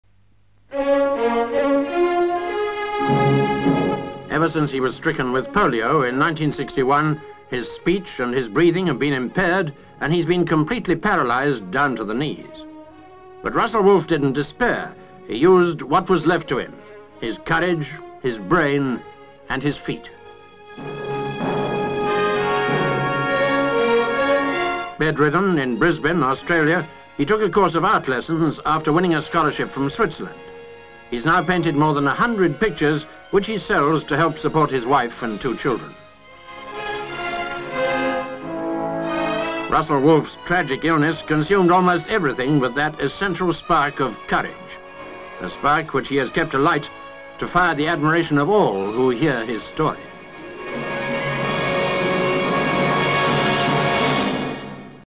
ONE MAN'S COURAGE
(27 Sep 1965) Russell Wolf was paralysed in 1961 with polio. Bedridden, in Brisbane, Australia, he took a course of art lessons after winning a scholarship from Switzerland. He has now painted more than 100 pictures which he sells to support his wife and two children.